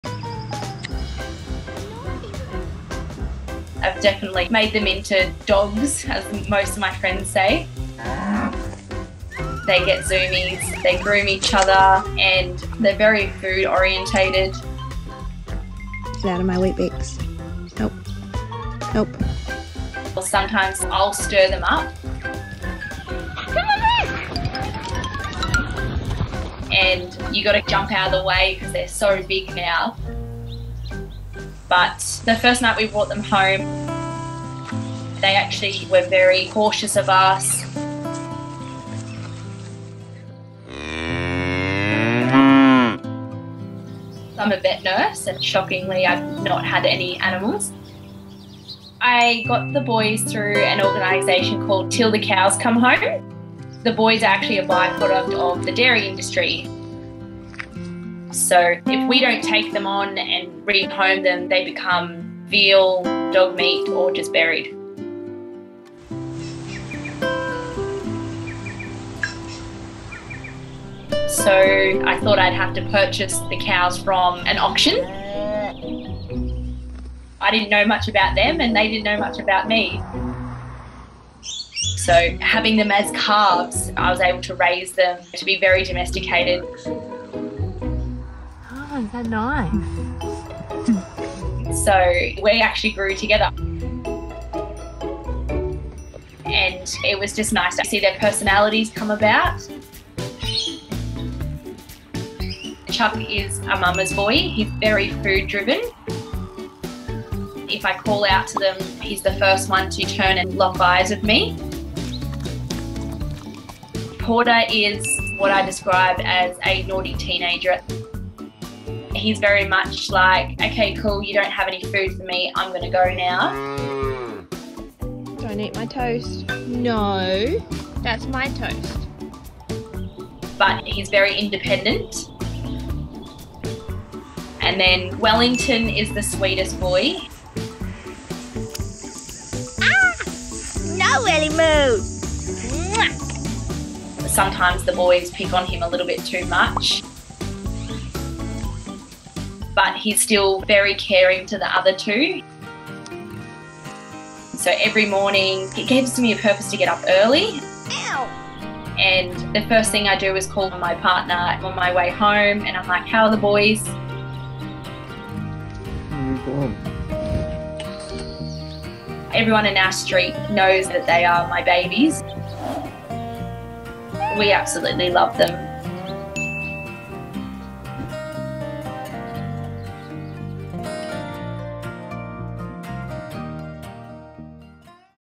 When these cows joined her family. She was never the same | Cuddle Buddies
Alicia adopted Chuck, Wellington and Porter when they were only babies. If she had not brought them to her farm, they would have been slaughtered. But it didn;t take long for them to find their feet. They each have their own unique personalities and Alicia sees them as her 3 little puppies. They love zooming around the farm and getting up to mischief. She can’t imagine her life without them.